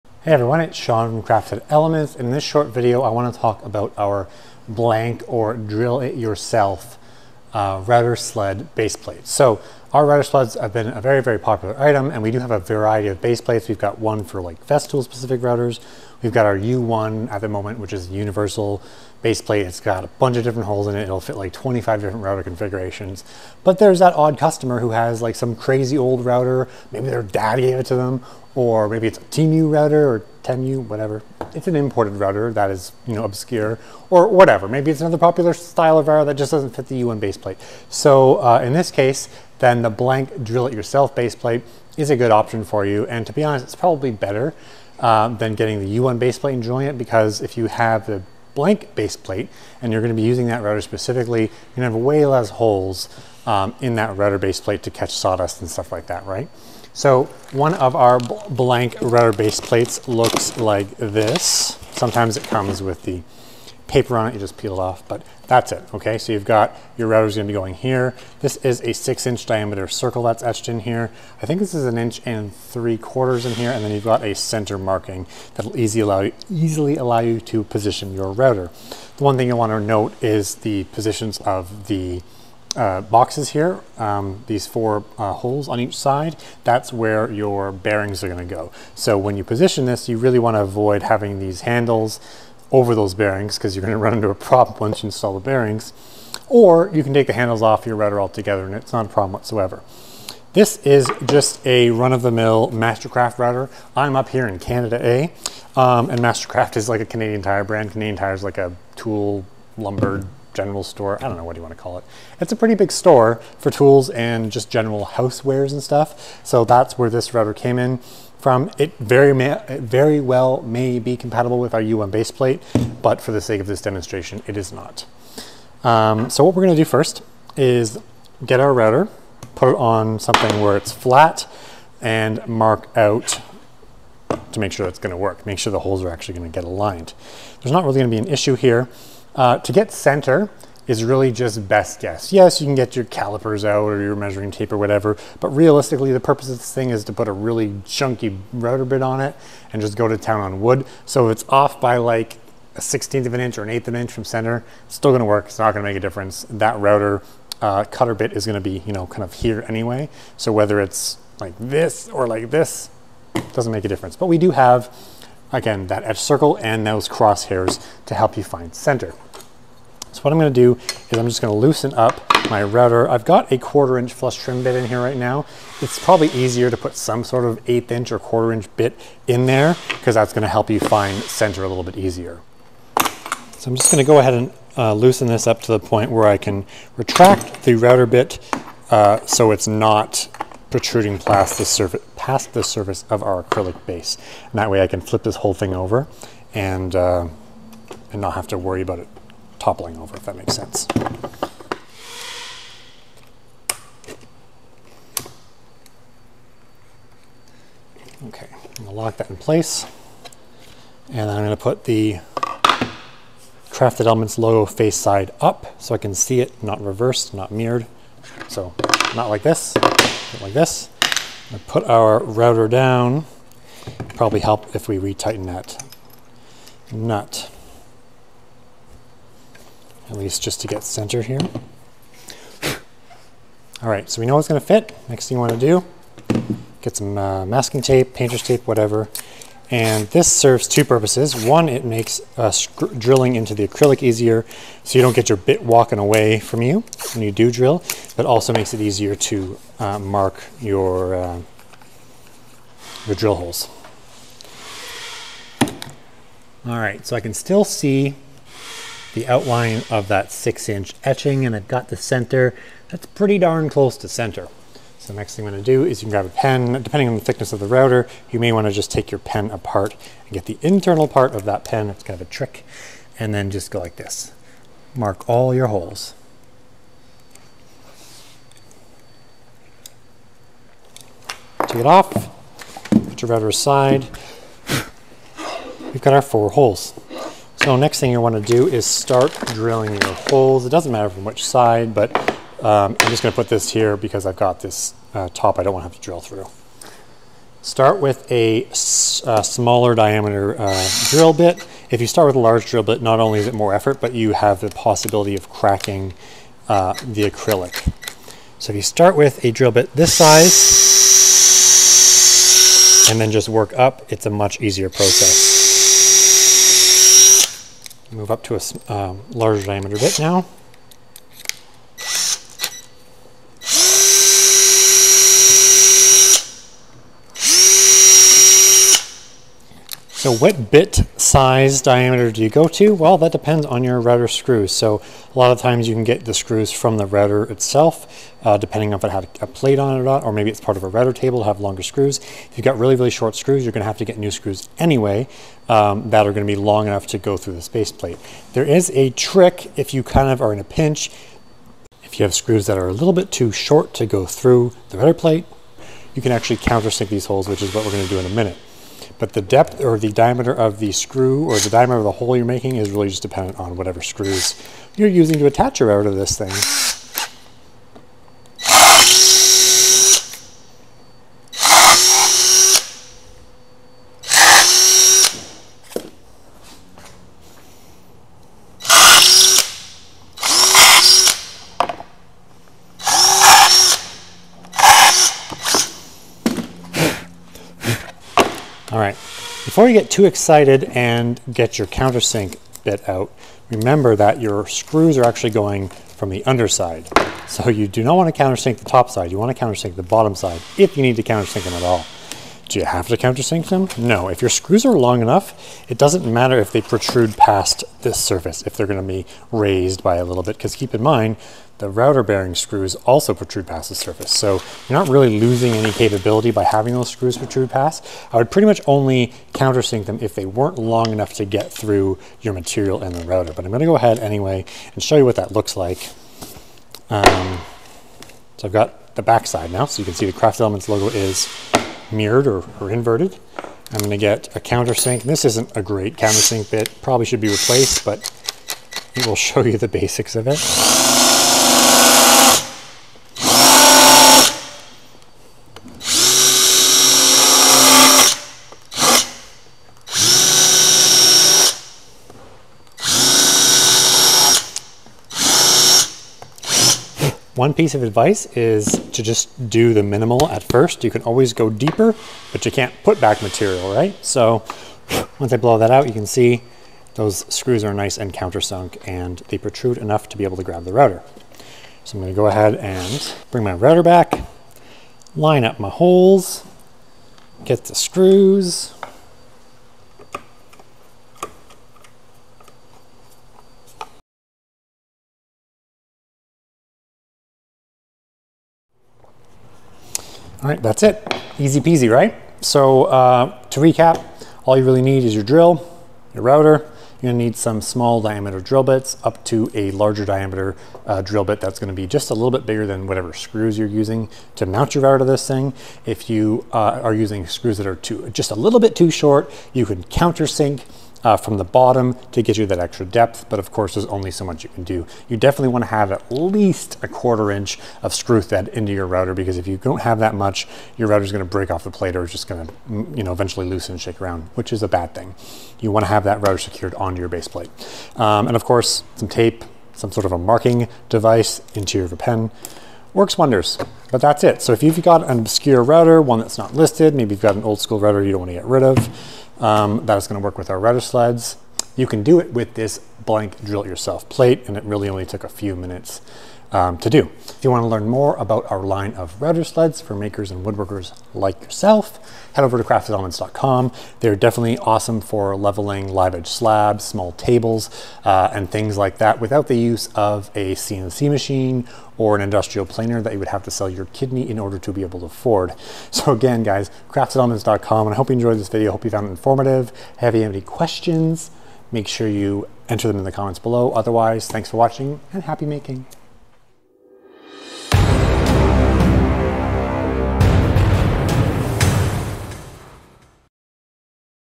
Drilling & Mounting Your Router To Our Blank Router Sled Baseplate - Crafted Elements
This router base is identical to the universal router base plate except there are no pre-drilled holes for mounting your router. Use this base if you have a router incompatible (or just not listed) with our other router bases. Drilling your own holes is easy, simply apply some masking tape to the plate, center the router, mark the hole locations, put the router aside, then drill. Start with a 1/8" bit to drill a pilot hole then progressively move up in bit sizes until you get to the required hole diameter to accommodate your mounting screws. This will help to avoid cracking the acrylic. You can also use a countersink bit to ensure your screws sit level with the plate.

Recommended Product Links & Resources
1/4" Flush Trim Bit 1: CE HP 1/4" Flush Trim Bit